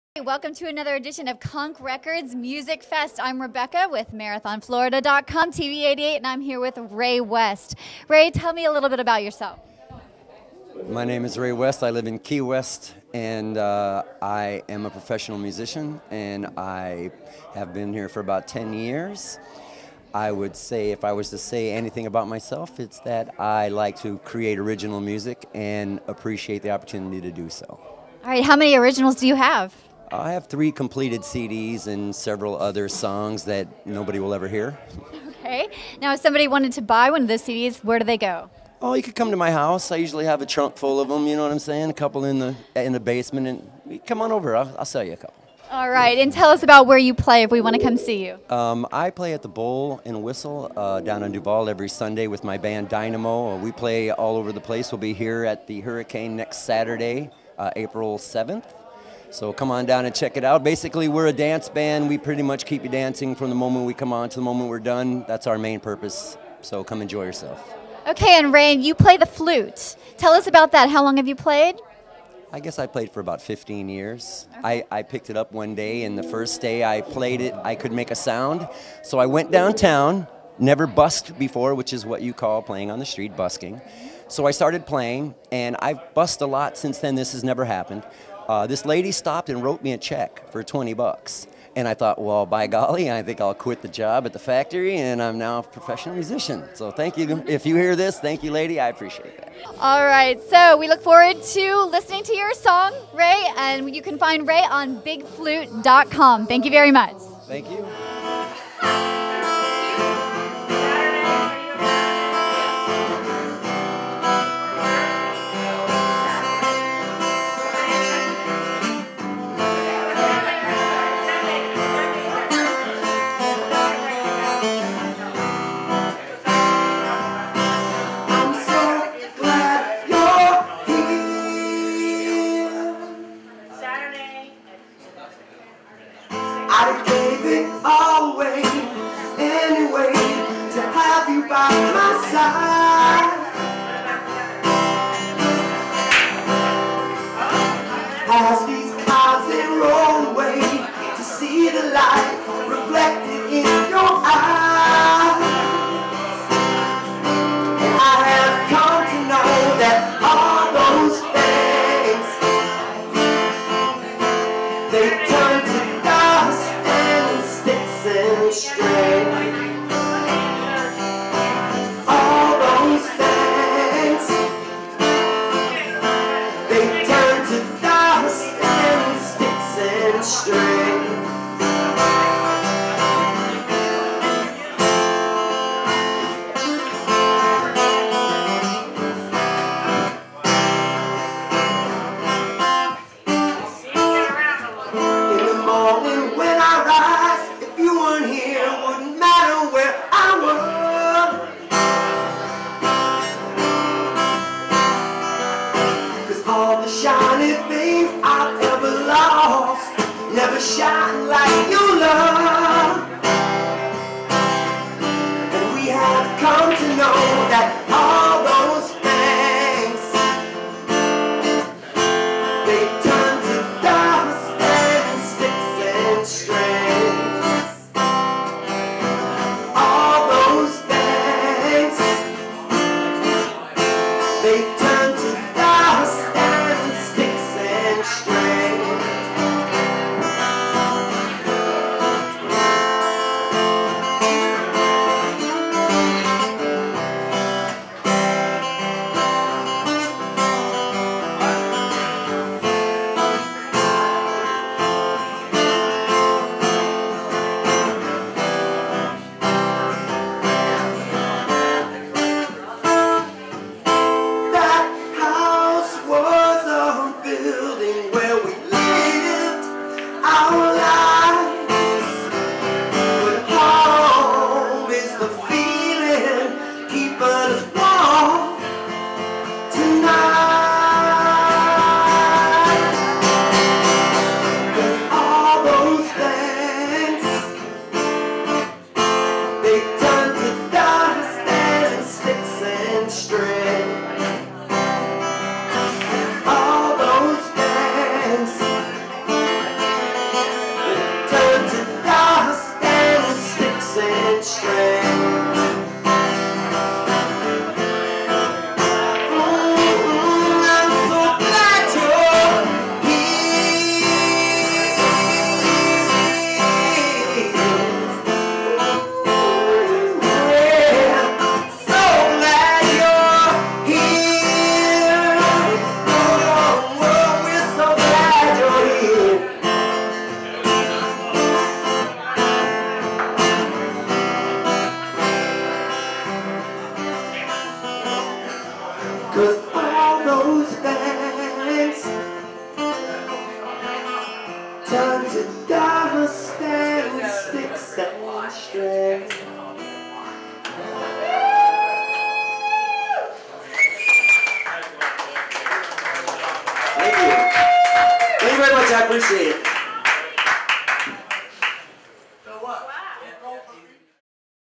Ray West - Conch Records Music Video Fest, 2018 - Hurricane Grille
Ray West Live at the Conch Records Music Video Fest which was held on March 30, 2018 at The Hurricane Grille..